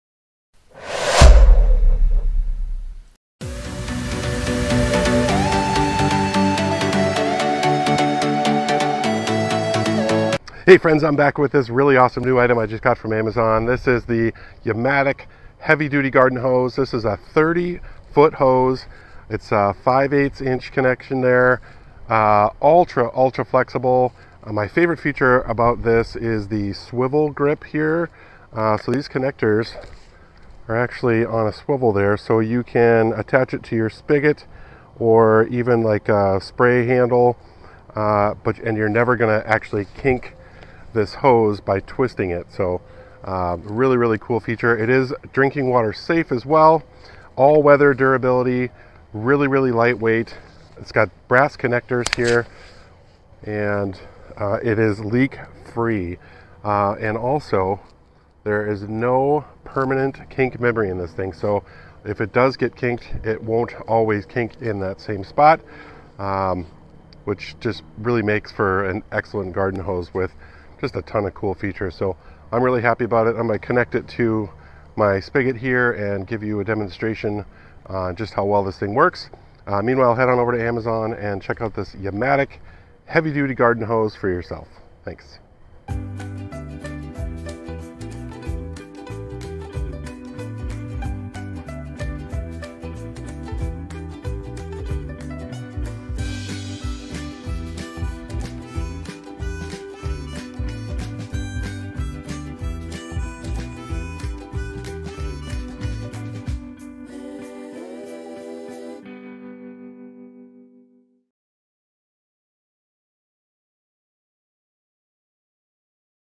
IS THE YAMATIC Water Drinking Safe Garden Hose WORTH IT? - Buyer's Guide
In this vlog, You can see the YAMATIC Water Drinking Safe Garden Hose Working
Happy Gardening!